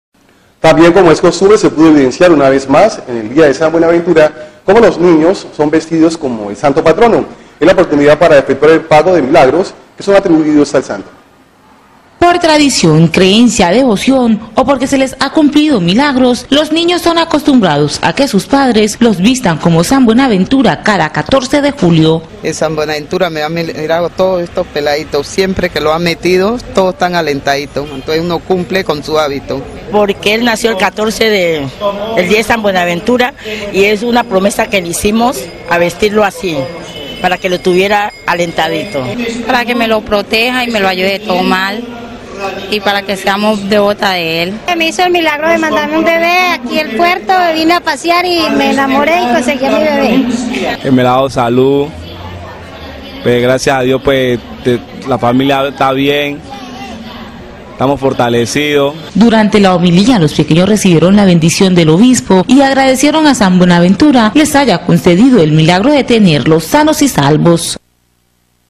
Los niños se vistieron como San Buenaventura continuando con la tradición
TITULO:Los niños se vistieron como San Buenaventura continuando con la Tradición.
SINOPSIS:Los niños se vistieron como San Buenaventura continuando con la Tradición del Pacífico Sur Colombiano.
SERIE:Prácticas Tradicionales
AUTOR:Soy de Buenaventura
REALIZADOR:Soy de Buenaventura
FECHA:15 de Julio de 2010